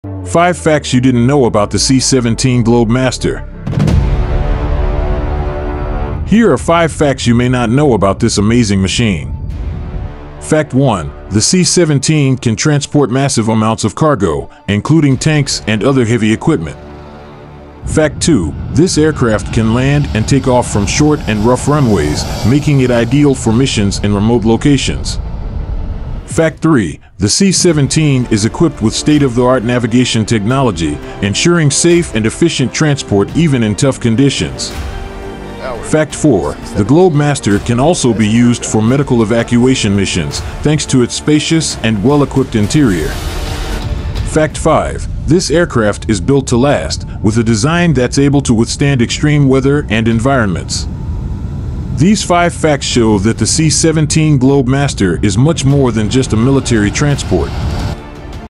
5 Facts You Didn't Know About the C-17 Globe Master
5 Facts You Didn't Know About the C-17 Globe Master
Here are 5 facts you may not know about this amazing machine.
Fact 1: The C-17 can transport massive amounts of cargo, including tanks and other heavy equipment.
Fact 2: This aircraft can land and take off from short and rough runways, making it ideal for missions in remote locations.
Fact 3: The C-17 is equipped with state-of-the-art navigation technology, ensuring safe and efficient transport even in tough conditions.
Fact 4: The Globe Master can also be used for medical evacuation missions, thanks to its spacious and well-equipped interior.
Fact 5: This aircraft is built to last, with a design that's able to withstand extreme weather and environments.
These 5 facts show that the C-17 Globe Master is much more than just a military transport.